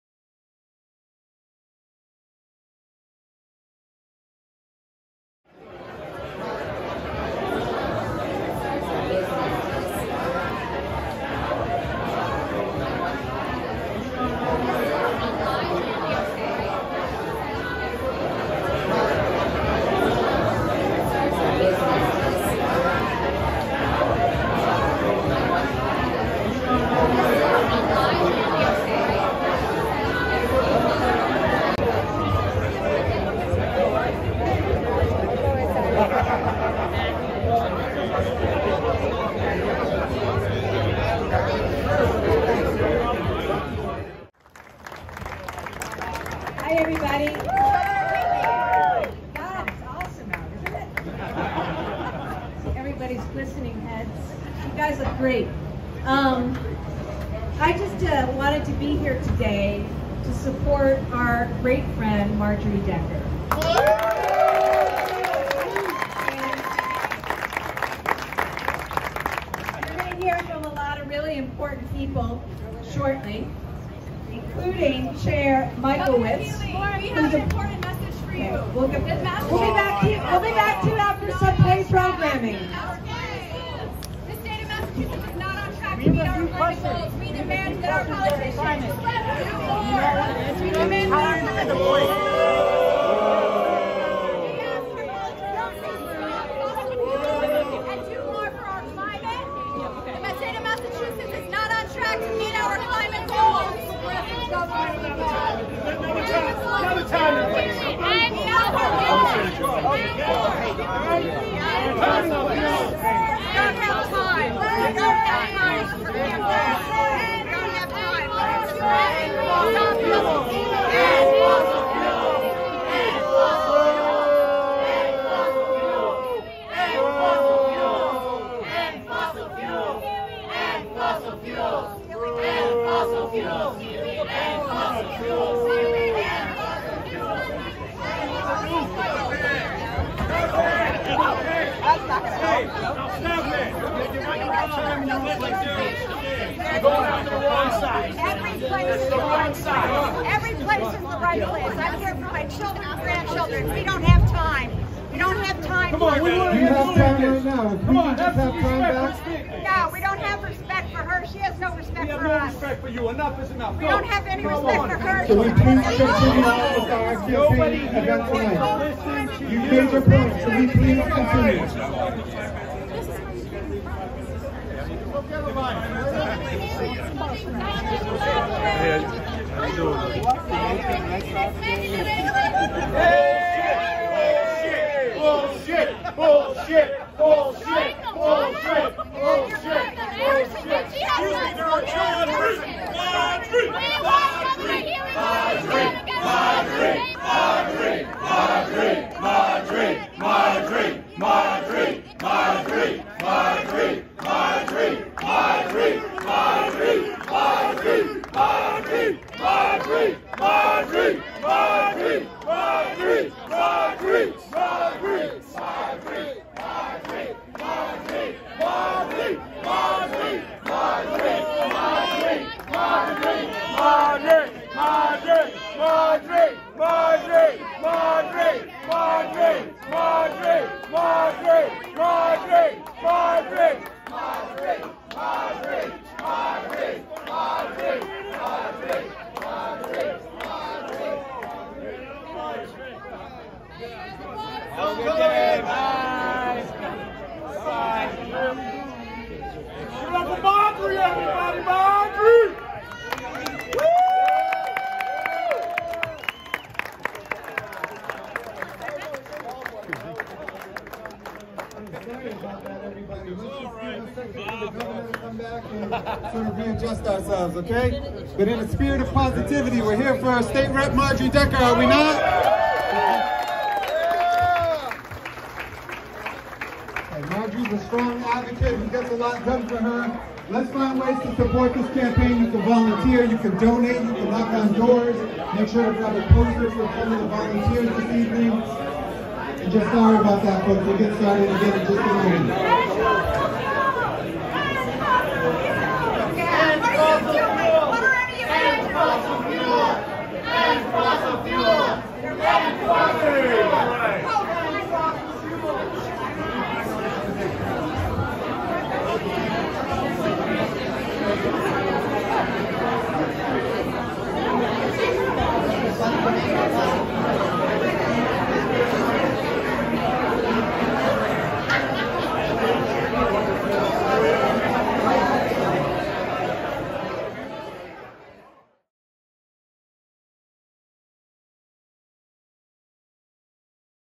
"We'll be back with some great programming"
The Boston-based group Climate Defiance confronted Massachusetts Governor Maura Healey as she supported Representative Marjorie Decker at a fundraising party in Cambridge. It was an exceptionally hot day, in an exceptionally hot summer globally. Climate Defiance's main message, shouted above the crowd's attempt to silence them, was End Fossil Fuels.

A movie by Skip Schiel & Teeksa Photography on July 8, 2024.